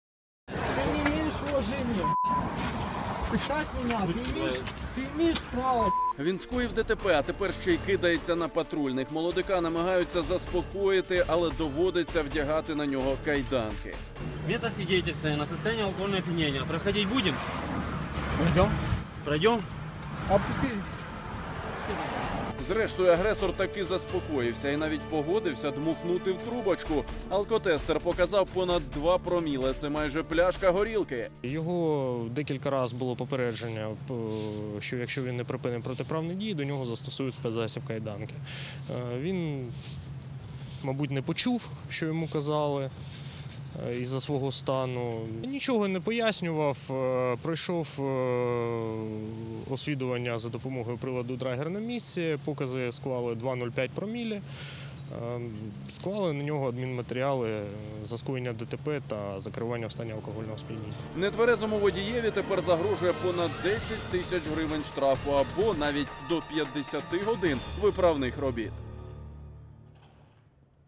У Миколаєві на 10 тисяч оштрафують нетверезого водія
Він скоїв ДТП, а тепер ще й кидається на патрульних. Зрештою агресор таки заспокоївся. Алкотестер показав, що чоловік випив майже пляшку горілки. Нетверезому водієві загрожує 10 тисяч штрафу або 50 годин виправних робіт